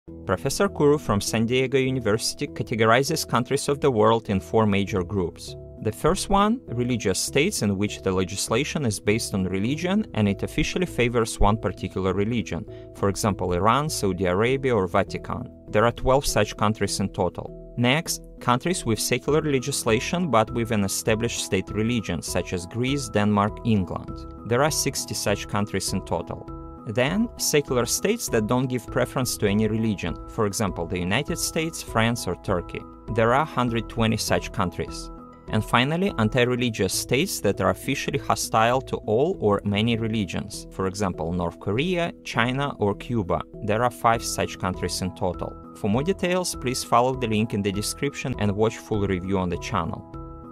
How many Anti-Religious states and Secular and Religious states are in the world?
Professor Kuru from San Diego University categorizes countries of the world in four main categories: religious states, states with secular legislation but with an established state religion, secular states, and anti-religious states.

Bibliography:
 Ahmet Kuru. 2009. Secularism and State Policies Toward Religion: The United States, France and Turkey. Cambridge University Press.
Recommended readings:
 Jacques Berlinerblau. 2013. How to be Secular A Call to Arms for Religious Freedom.
 Rajeev Bhargava “The Distinctiveness of Indian Secularism.” In The Future of Secularism, edited by T.N. Srinivasan. Delhi: Oxford University Press.
 Calhoun, Craig, Mark Juergensmeyer, and Jonathan VanAntwerpen, eds. 2011. Rethinking Secularism, Oxford University Press.
 Copson, Andrew. 2019. Secularism: A Very Short Introduction. Oxford UP.
  “Secularism in France,” by Amelie Barras, in Oxford Handbook of Secularism, edited by Zuckerman and Shook
 Disenchanting India: Organized Rationalism and Criticism of Religion in India. Johannes Quack
 Zuckerman, Phil 2008. Society Without God. New York, NY: New York University Press.